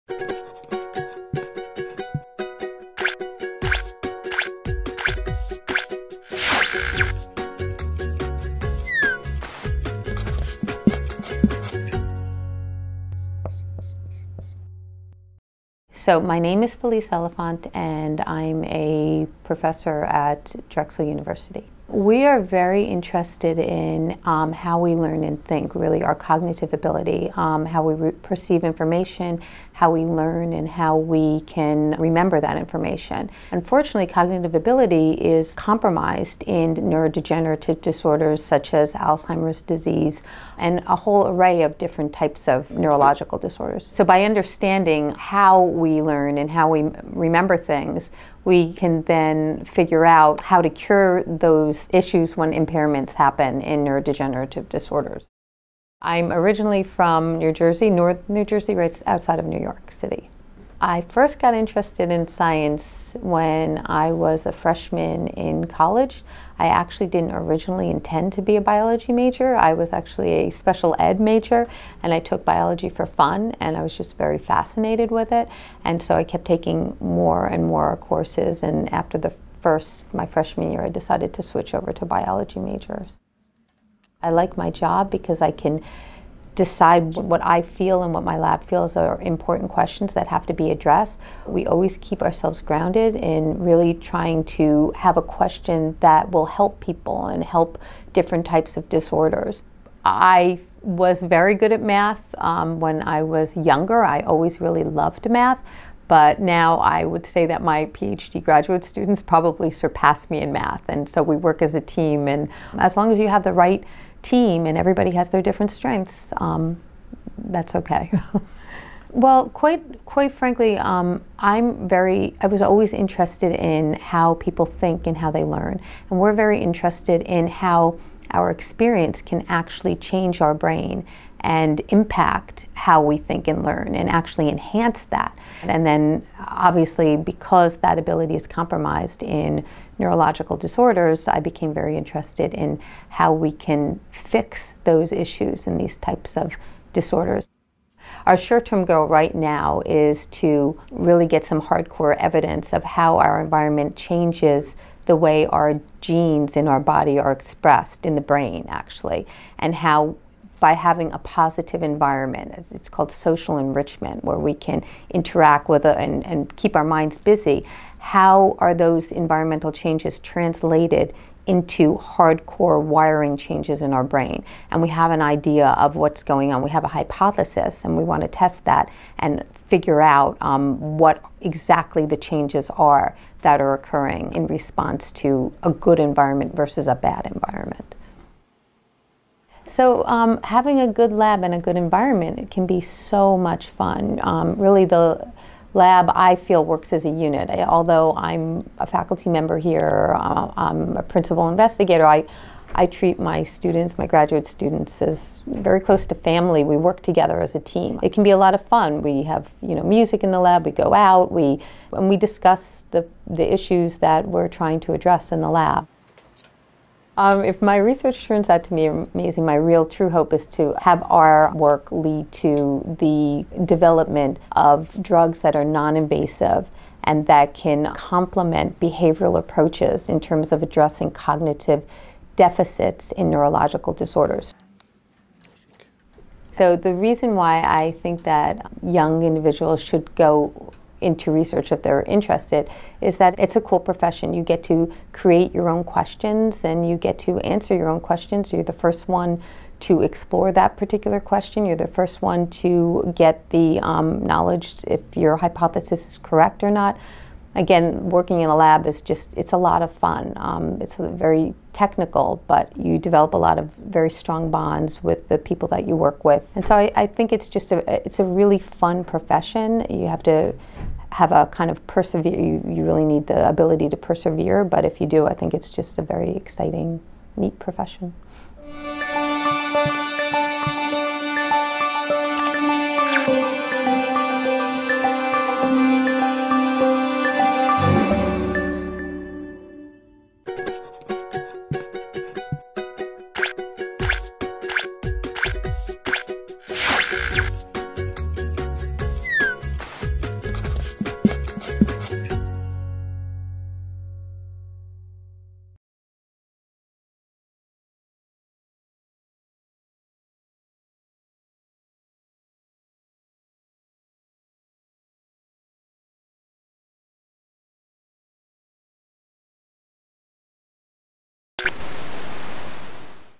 Medical Research. Is It The Profession For You?
Meet Felice Elefant, principal investigator & professor in the Elefant Lab at Drexel University. Felice's research involves the neuroscience of learning, with the goal of learning more about neurodegenerative diseases.